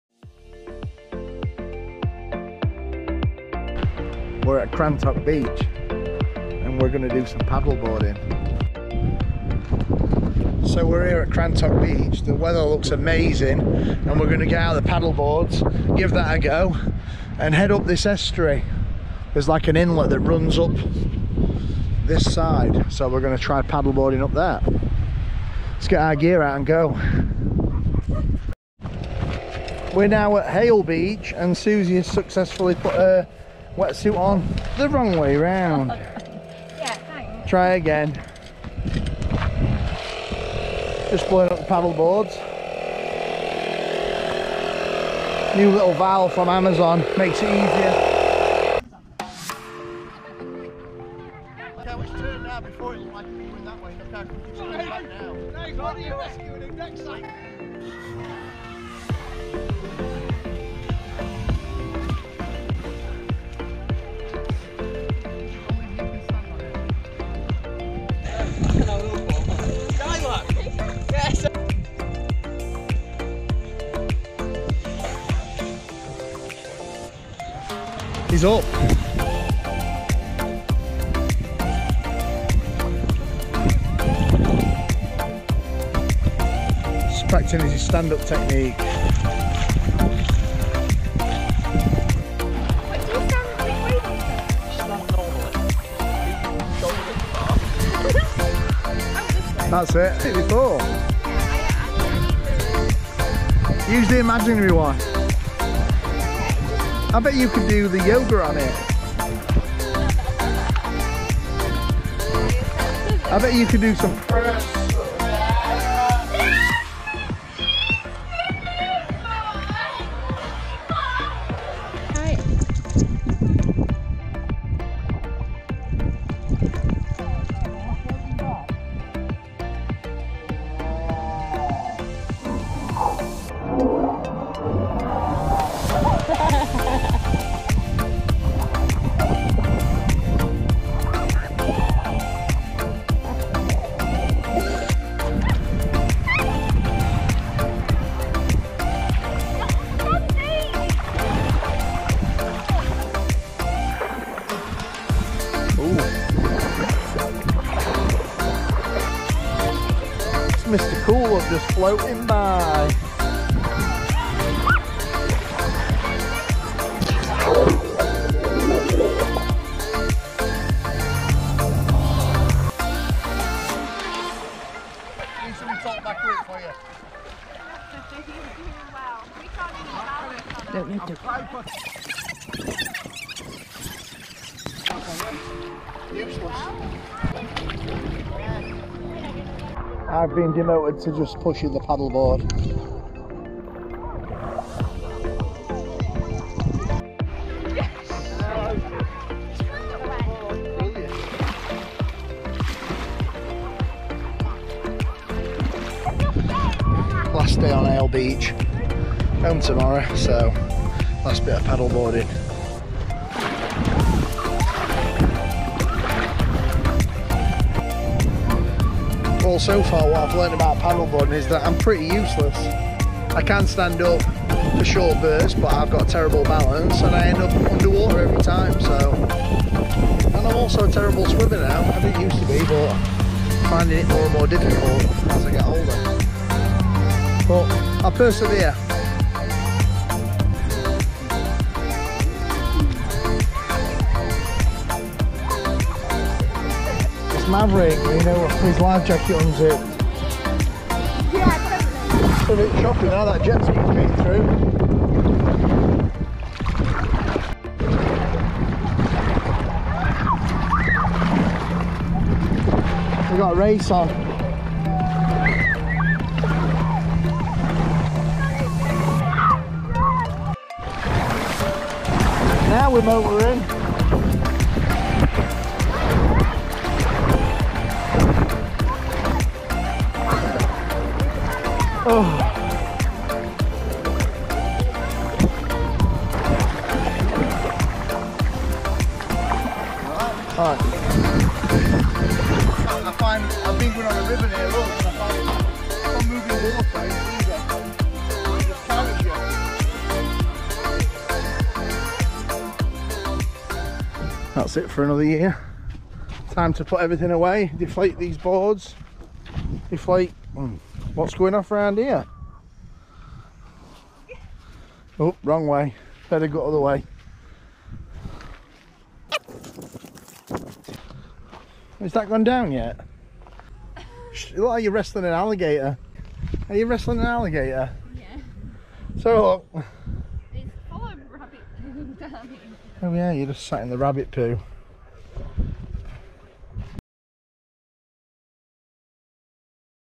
Hayle beach - Cornwall
Last day in Cornwall at hayle beach,  crantock was super busy so quick change of plan . we decided to hit  Hayle Beach in Cornwall which is great for the whole family and perfect forvthe paddle boards